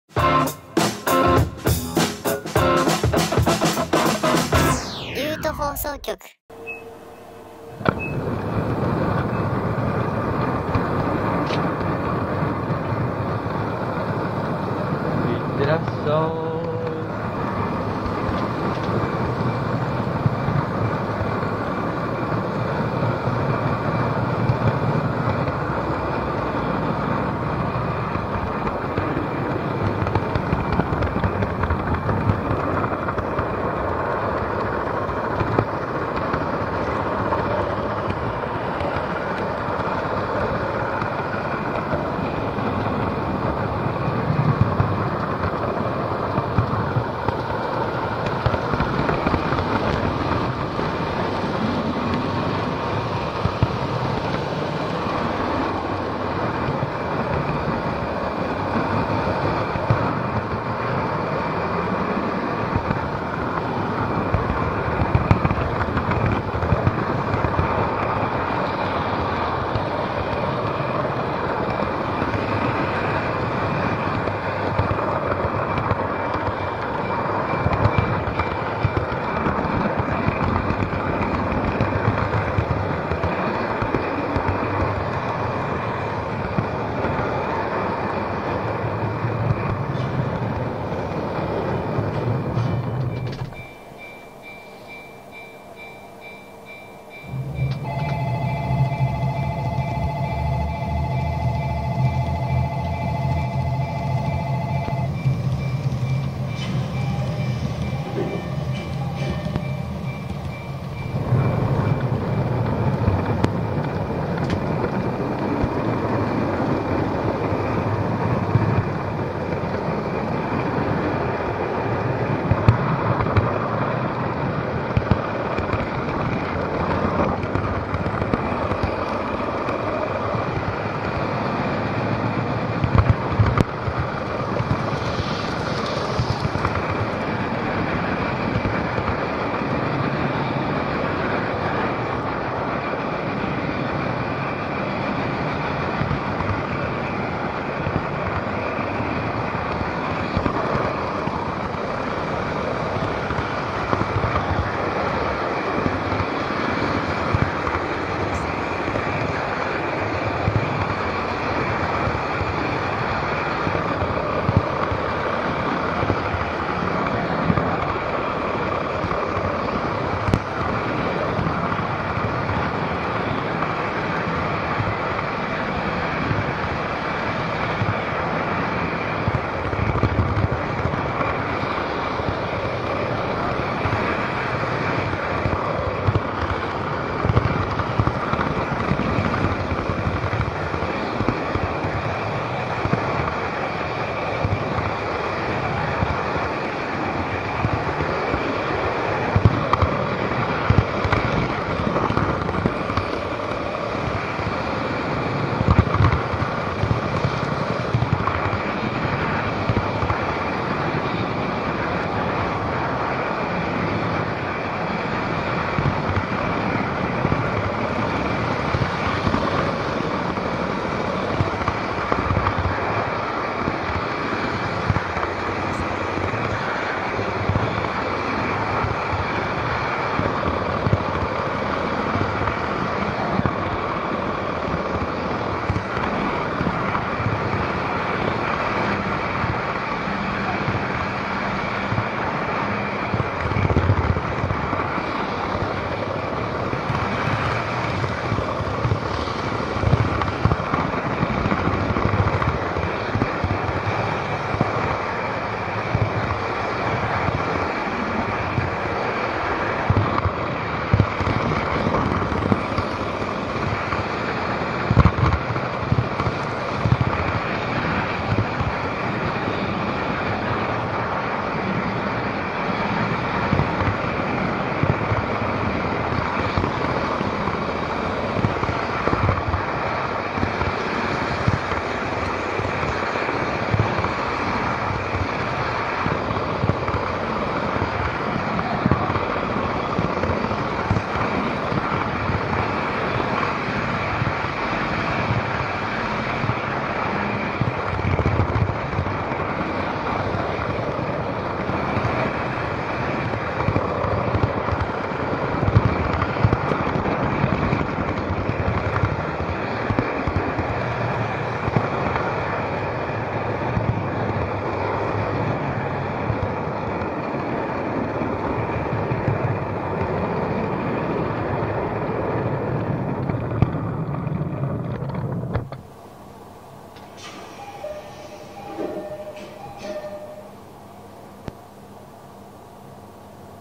【前面展望】Nゲージ 内回り線 2019/2/5
久しぶりの前面展望の動画です。
今回は内回り線の前面展望です。